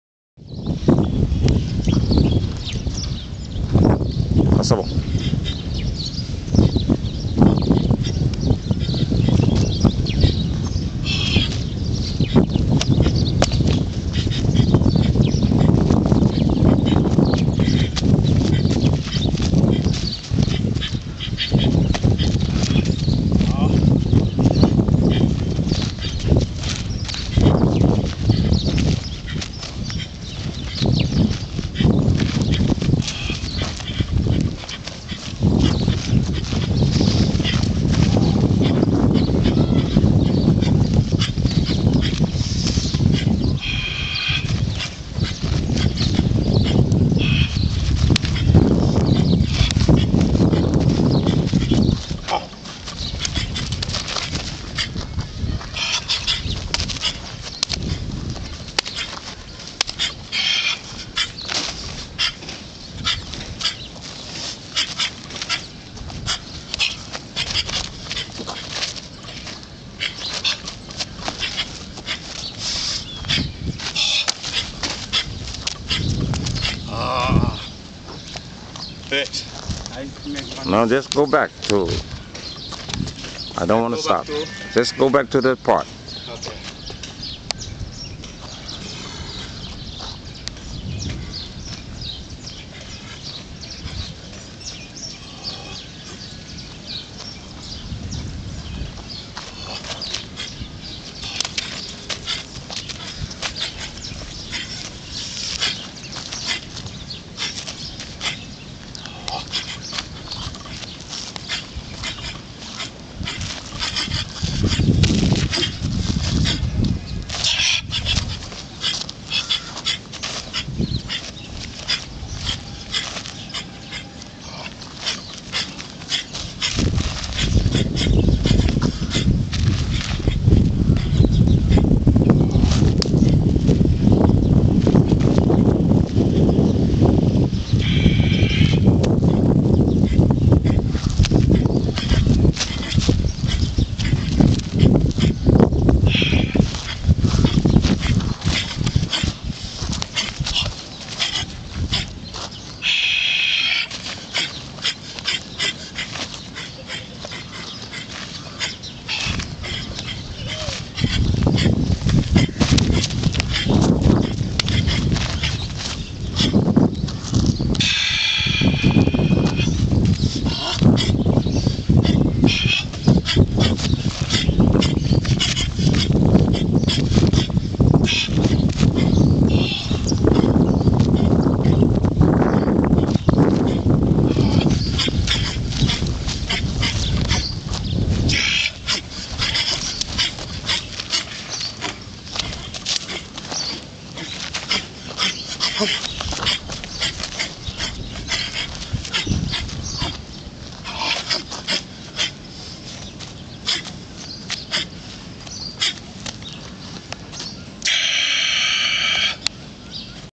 form # 2.MOV Tiger/Praying Mantis Boxing
Shaykh Al-Mulakim teaches Tiger Praying Mantis Kung Fu in West Africa